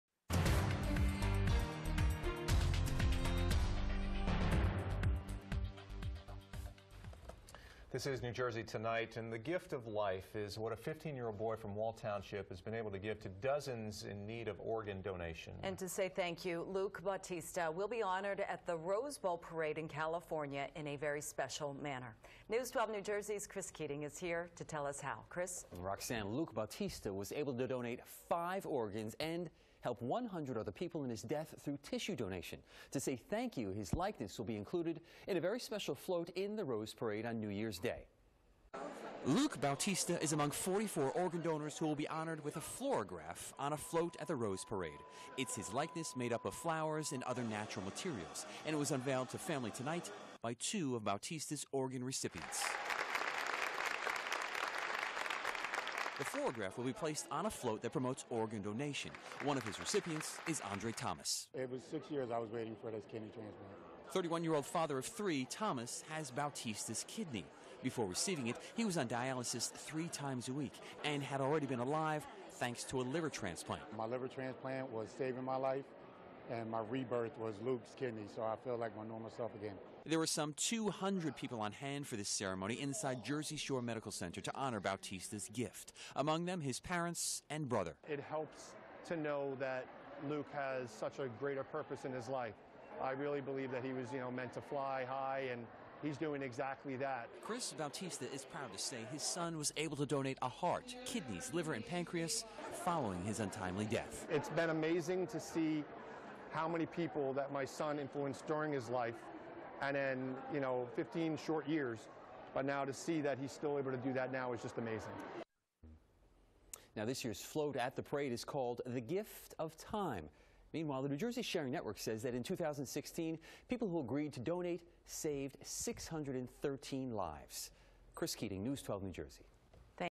Jersey Shore University Medical Center - News 12 NJ - The Gift of Time
At the age of 15, Luke Bautista of Wall passed away. But in that death, his organs helped save the lives of 5 people. He will be honored with his floragraph (a portrait made completely of natural and organic materials) image displayed on the Donate Life float in the Rose Bowl parade in January. The floragraph was unveiled by two of his organ recipients, and his family then added the finishing touches to it during a very moving program at Jersey Shore University Medical Center. Luke is one of 44 organ donors across the country selected to have his image created as a floragraph and have it included on the Donate Life float.